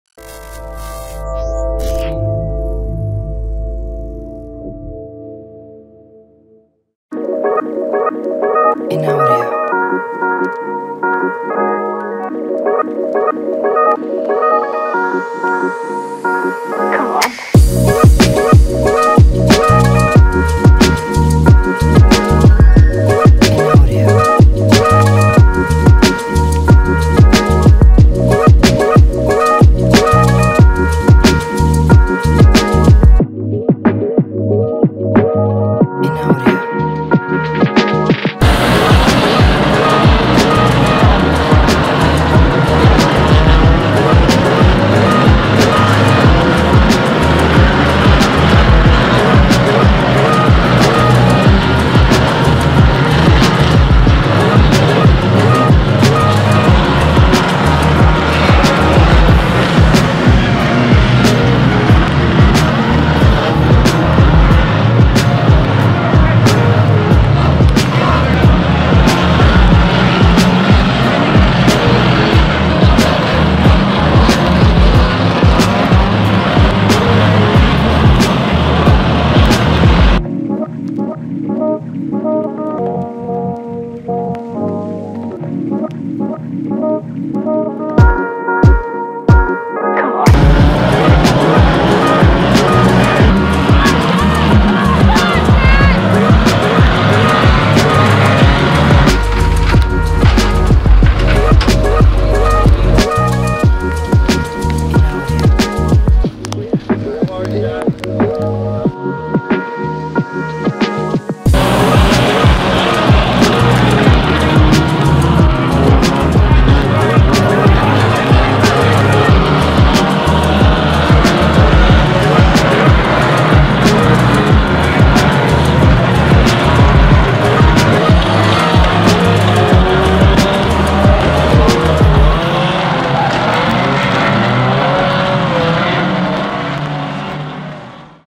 Ipswich 2021 Unlimited Banger World Final Impact Videos Edit
A short edit of a selection of our footage from the 2021 Unlimited Bangers World Final at Ipswich. This will be edited into the full version from Spedeworth TV so check them out for the full length version of the wettest Banger World Final in recent years, but there was some excellent driving skills and hard crashes too.